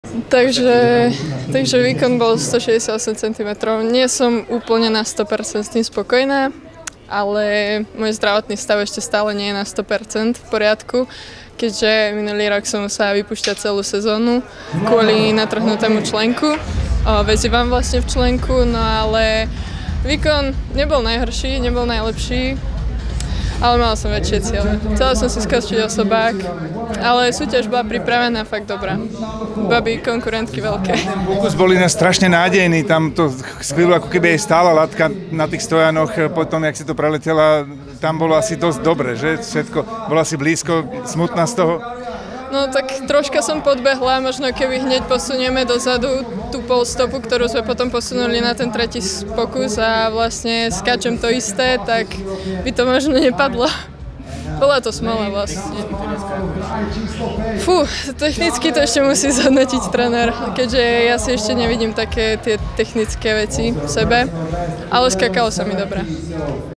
ME, U18, BB, 18.7.2024 Viktória Hudecová, výška kvalifikácia
Anna Viktória Hudecová sa nádejne pokúšala v kvalifikácii skoku do výšky na ME do 18 rokov o vyrovnanie osobného rekordu 171 cm. To sa jej však nepodarilo a tak si zapísala 168 cm a celkové 27. miesto.

OS: hlas k voľnému použitiu pre všetky médiá